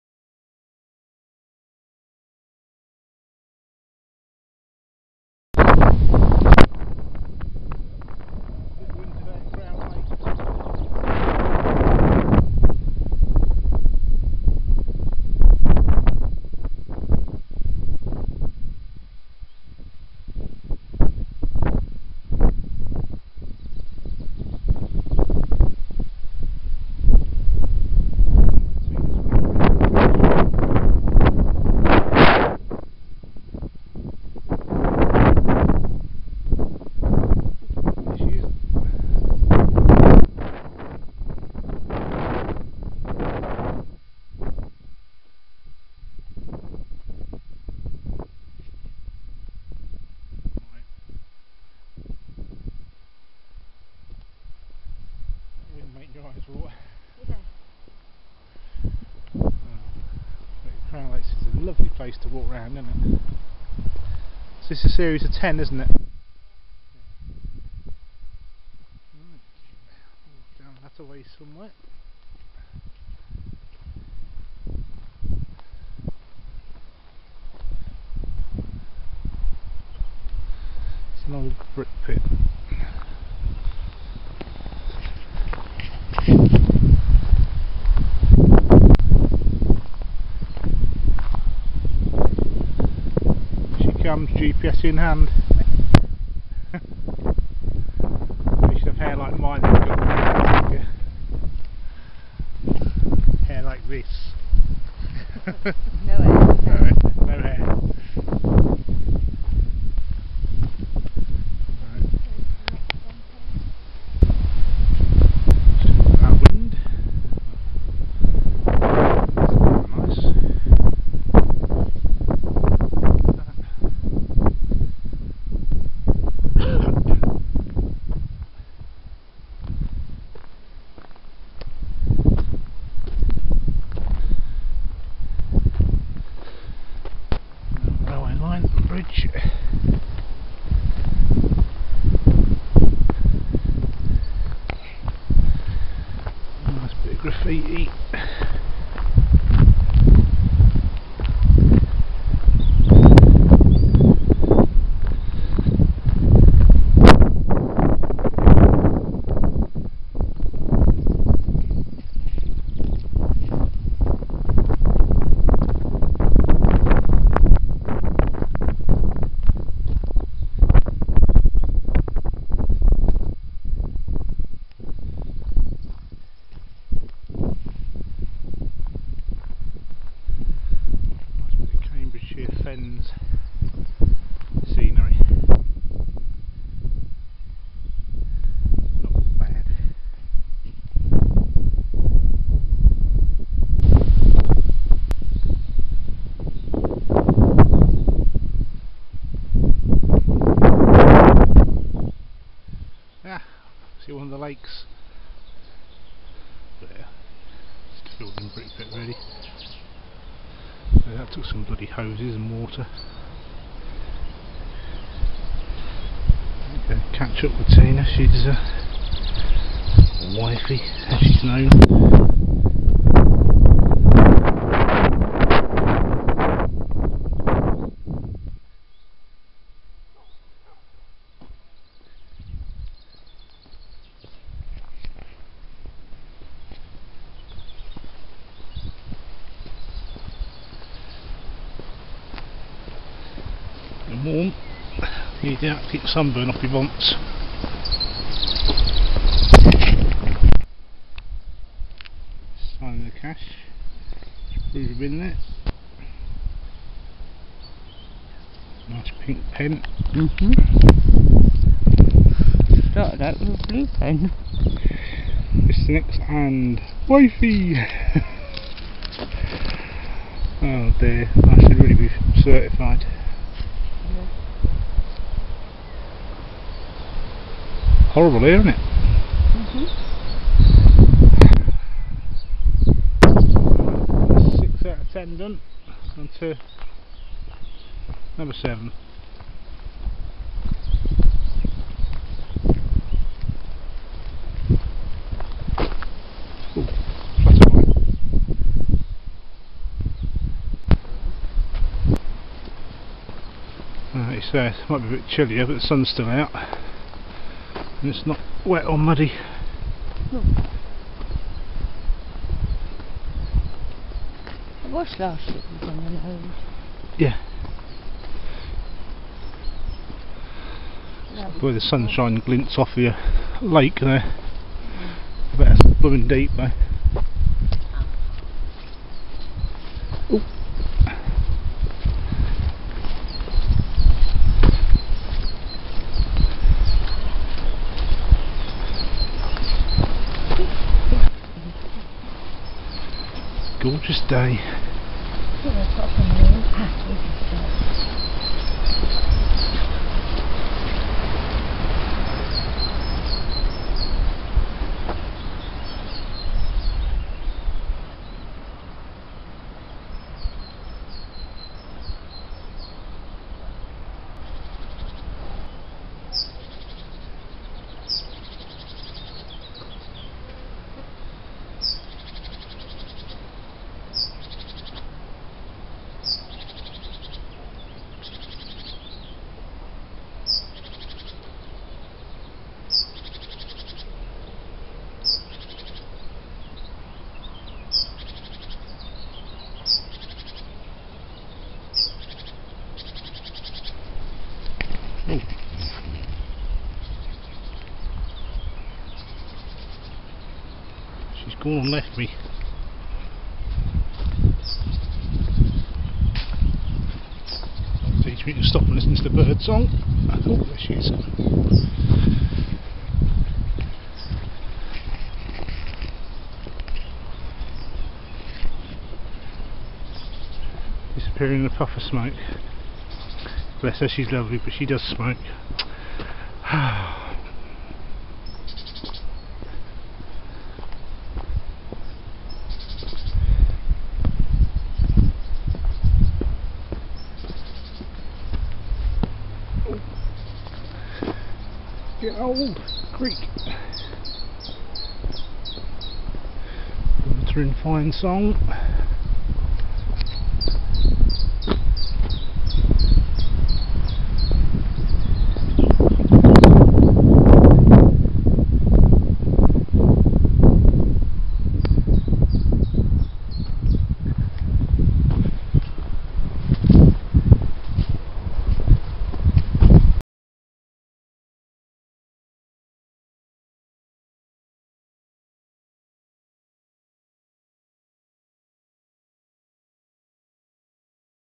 Geocaching - Crown Lakes, Farcet, Peterborough, England
around Crown Lakes, Farcet, Nr Peterborough, England. 27th April 2011. Sorry for the wind noise but it was a very blustery day.